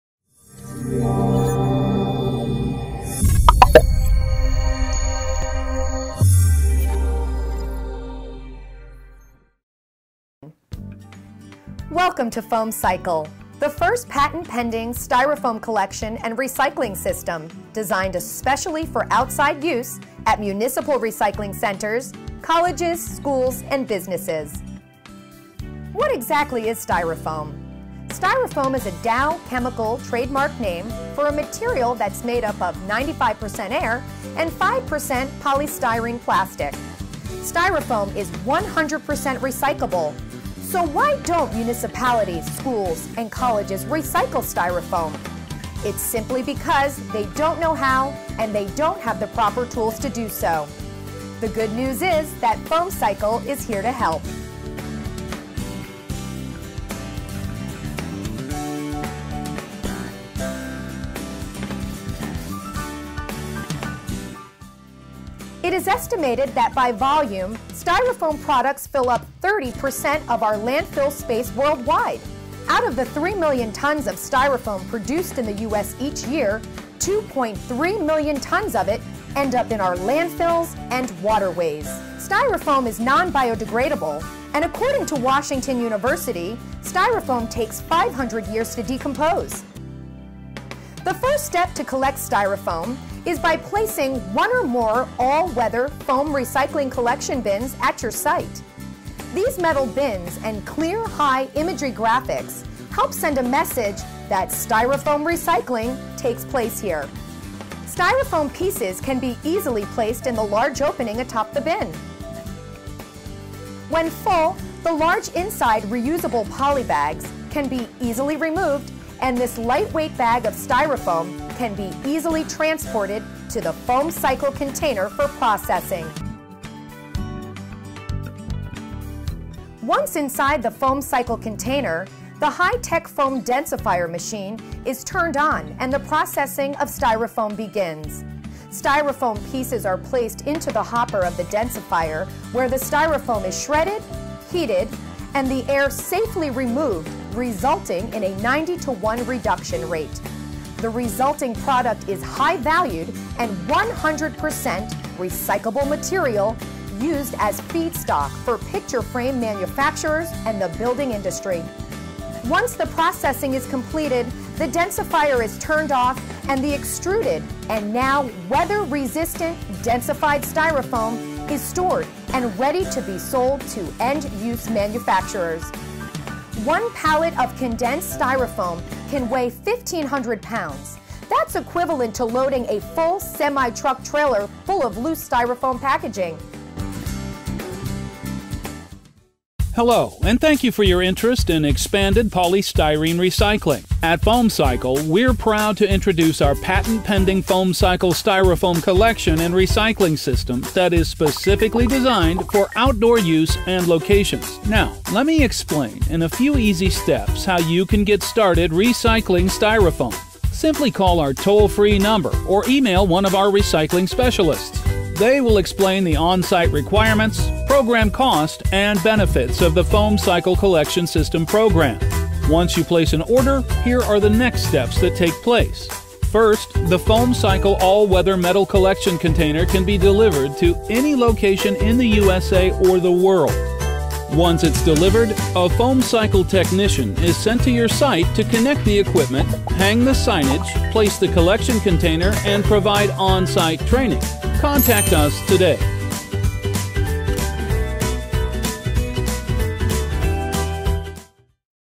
Foam Cycle Styrofoam Recycling Container System
RECYCLING STYROFOAM // FOAM CYCLE RECYCLING CONTAINER SYSTEM / Our innovative and patent-pending Foam Cycle container recycling system allows counties, municipalities, schools, colleges and businesses to collect, recycle, process and market Expanded Polystyrene (EPS #6 ) – also known as EPS Foam and Styrofoam.